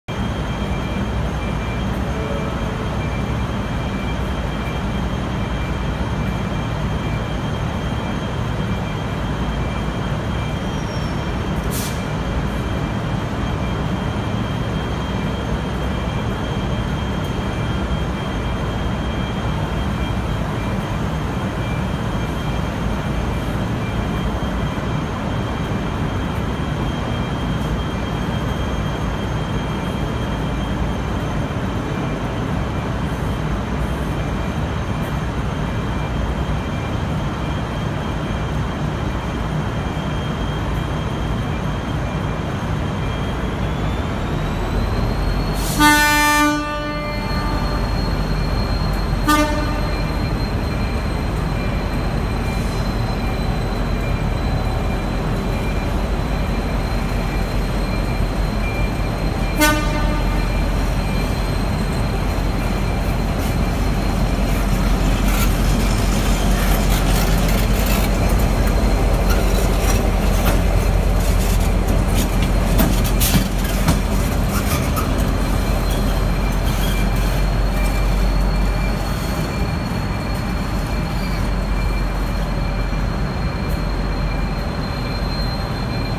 V/Line N472 City of Sale Low Note Horn
Train Loco: N472 City of Sale
Date: August 15 2017
Location: Southern Cross Station